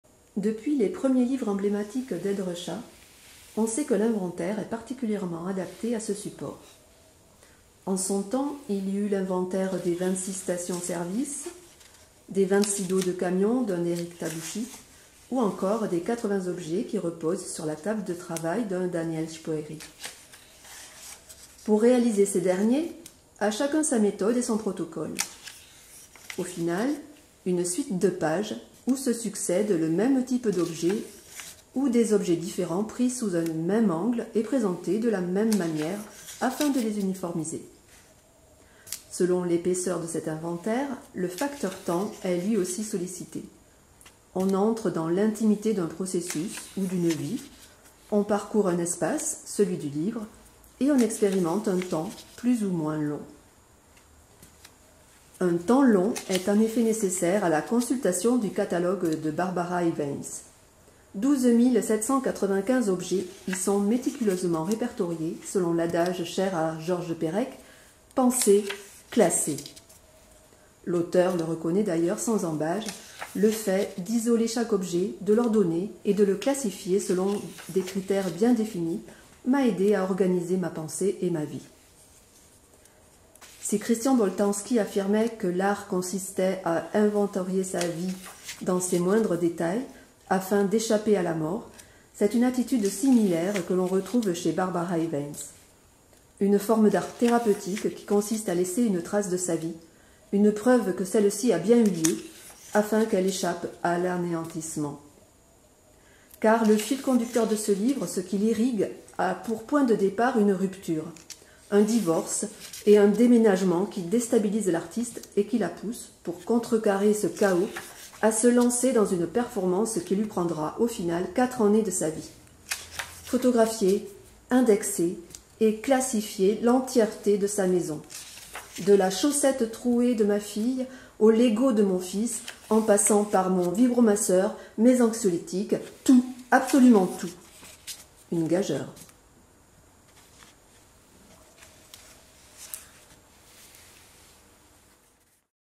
Le 26 du mois n°86 / Barbara Iweins
Chaque mois, la bibliothèque des Abattoirs vous présente un livre d’artiste issu de sa collection.
Découvrez "Katalog" © delpire & co, de Barbara Iweins, 2022